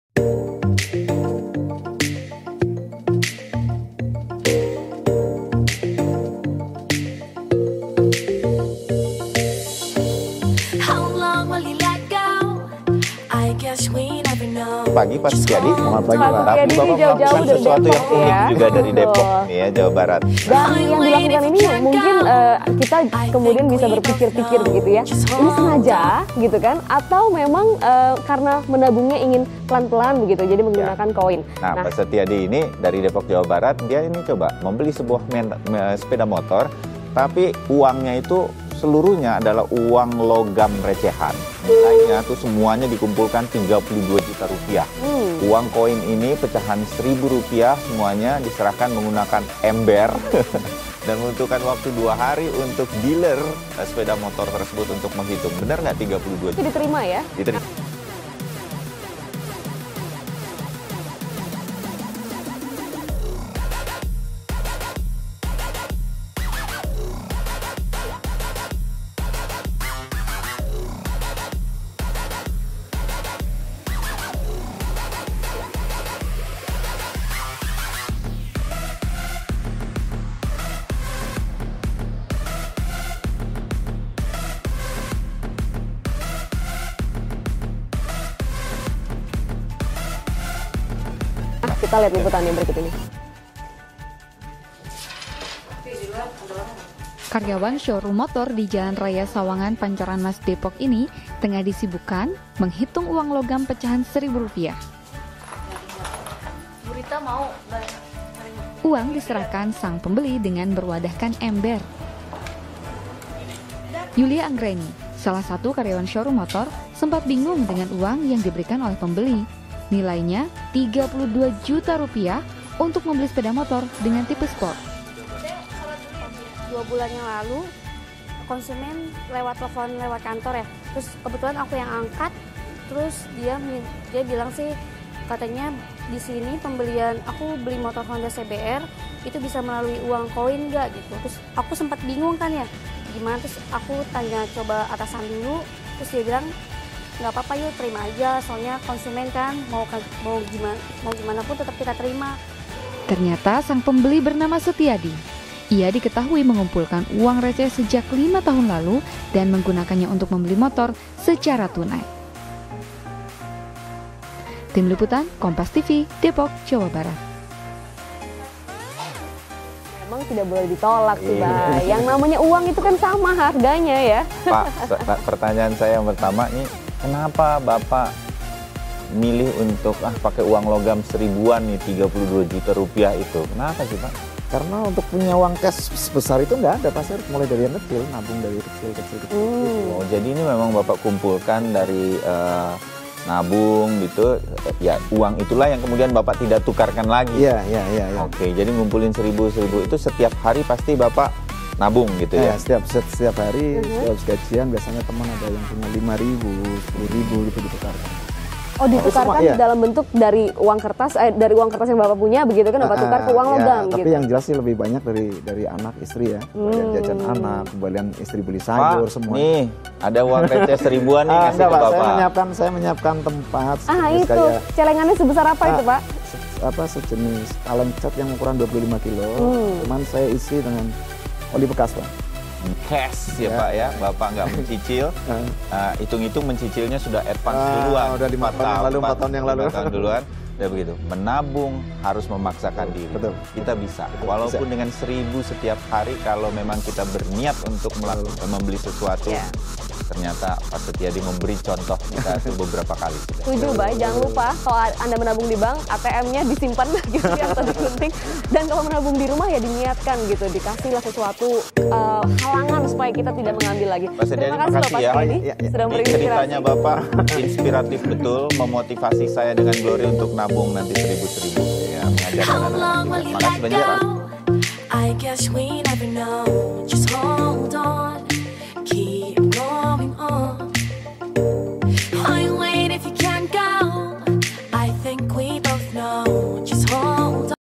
Beli Motor Pakai Uang Koin Jadi Viral Netizen
Setiadi, seorang konsumen Honda Motor Care, di Jalan Raya Sawangan, Depok, membeli Honda CBR150R Repsol dengan cara yang unik. Dia membayar motor sport yang dibandrol sebesar Rp 33,425 juta secara tunai dengan pecahan uang koin, yang dikumpulkan di sebuah ember dan dus.

Mia Sisilia, pegawai Honda Moto Care Depok mengatakan bahwa konsumen tersebut datang dengan membawa uang koin di dalam ember berukuran 25 kilogram (kg).

"Kemarin sales yang menangani itu Yulia Anggraini. Bapaknya itu datang membawa ember bekas cat berukuran 25 kg yang isinya uang koin semua dan satu dus berisikan 14 plastik

WOW AMAZING